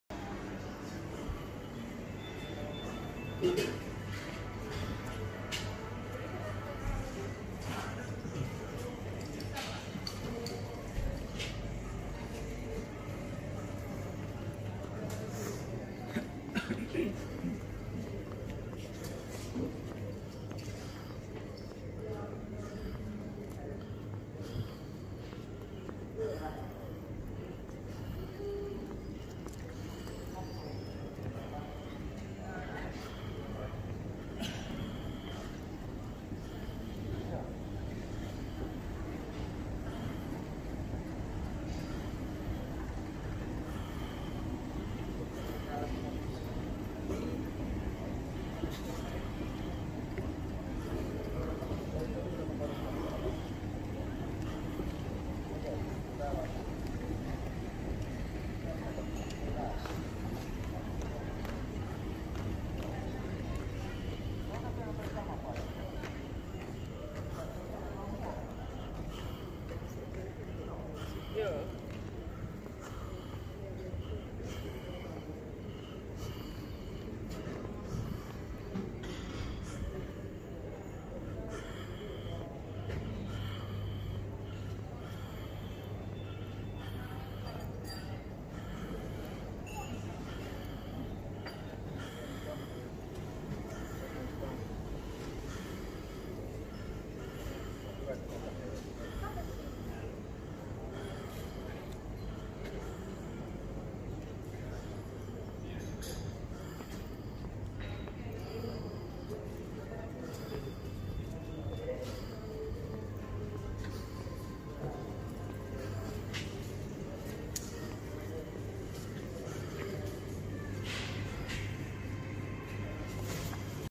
Tampere TAYS New Bldg Restaurants Store Pharmacy Samsung Galaxy S30 8K 5G Nets iPhone 12 Pro 4K 5G
Tampere TAYS New Buildings Restaurants
Pharmacy Convenience Store Coffee ☕ Shop
Anu purchases Postage Stamps in the
Convenience Store & Ice-Cream
Enjoy IGSP shares his adventures you tube
finland & Internet Tele Communications
you tube finland